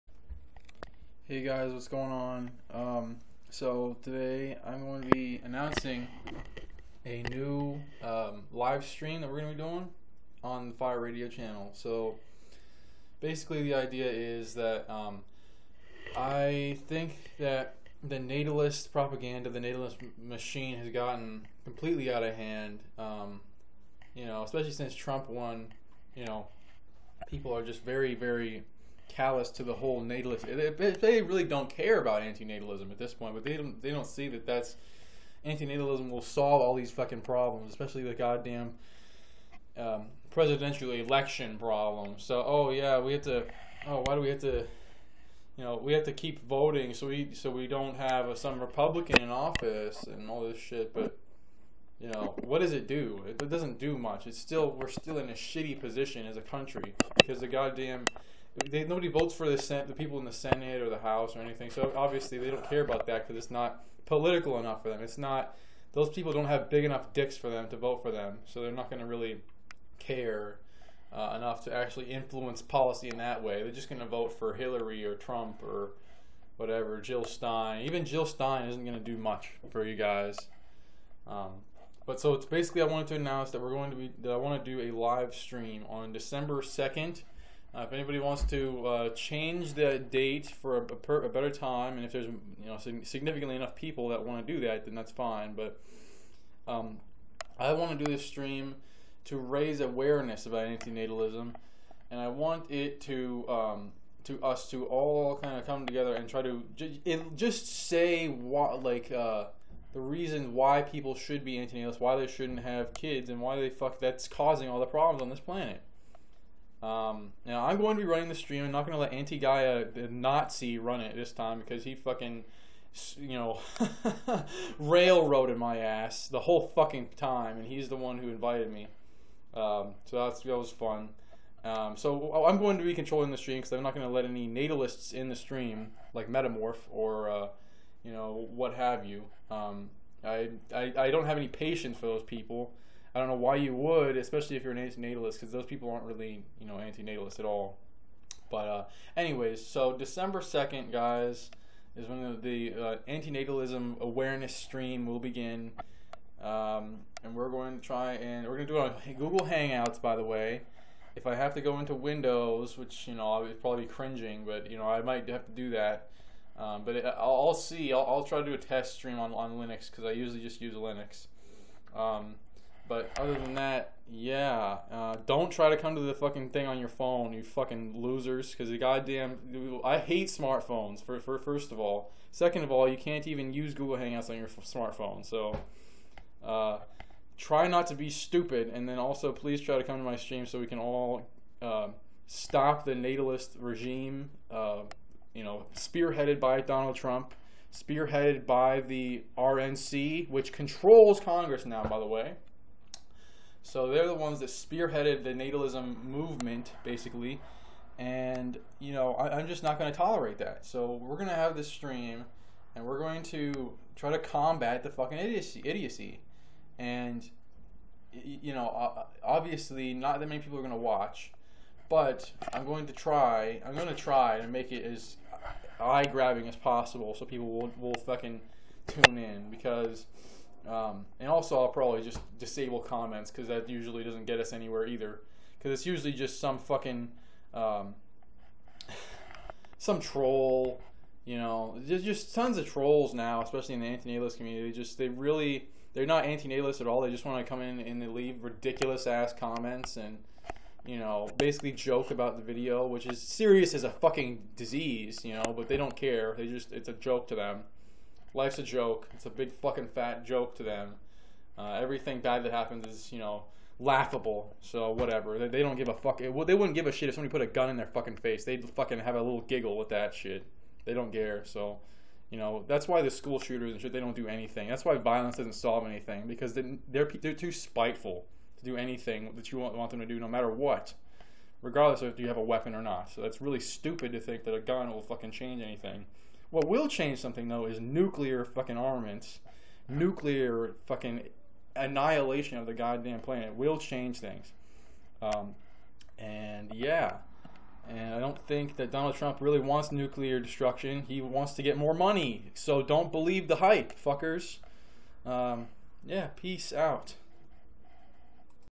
ON DEC. 2! 7pm PST Antinatalism Awareness Stream 2016
I'm not gonna stop fighting the natalist regime spearheaded by conservo-whores across America. I refuse. We WILL force the end of climate change and the end of human suffering and we don't give a fuck what the natalists have to say about it. No natalists will be allowed in the stream whatsoever. This isn't the time to play devil's advocate when a devil is running the country already, fucking morons!